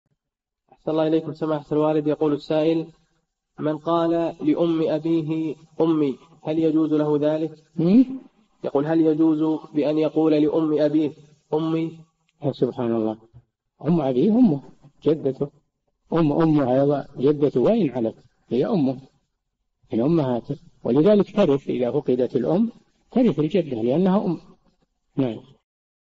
[1282- 4717] هل يجوز للشخص أن يقول لأم أبيه أمي؟ - الشيخ صالح الفوزان
"[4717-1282] من قال لأم أبيه أمي هل يجوز له ذلك؟
الجَوَابُ: يا سبحان الله! أم أبيه أمه جدته، أم أمه أيضاً جدته وإن علت فهي أمه، ولذلك إذا فقدت الأم ترث الجدة؛ لأنها أم.

للاشتراك في قناة الشيخ صالح الفوزان
فتاوى المجالس في تفسير المفصل

للاستفسار والإقتراحات والدعم
رئيس الفريق
لمشروع كبار العلماء